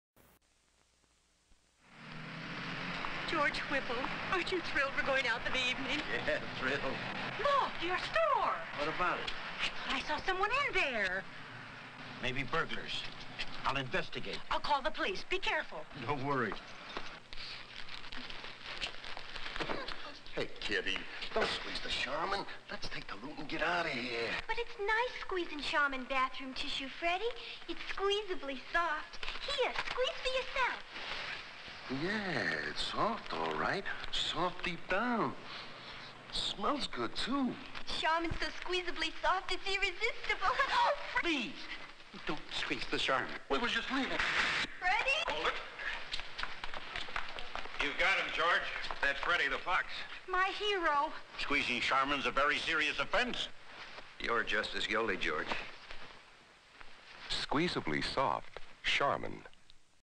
Charmin TV commercial
Funny commercial where burglars break into the store.